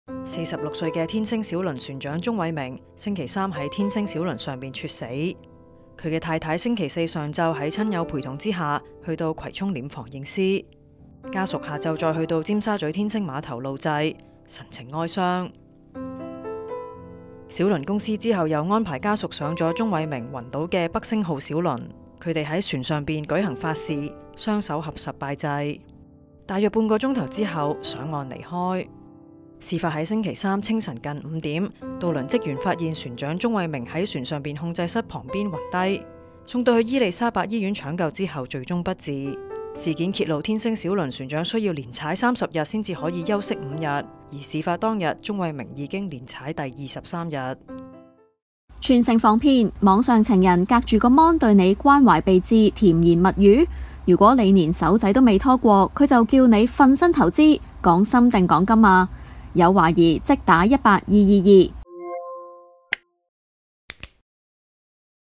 船長猝死｜遺孀悲痛認屍 擬下午天星碼頭路祭｜01新聞｜突發｜天星小輪
天星小輪46歲船長鍾偉明，昨日（3日）返通宵更期間暈倒渡輪上，送院搶救不治。事件揭示船長連踩23天工作，家人質疑猝死為疲累所致。死者遺孀在多名親友陪同下，今日上午到殮房辦理認屍手續，預計下午會到天星小輪碼頭拜祭。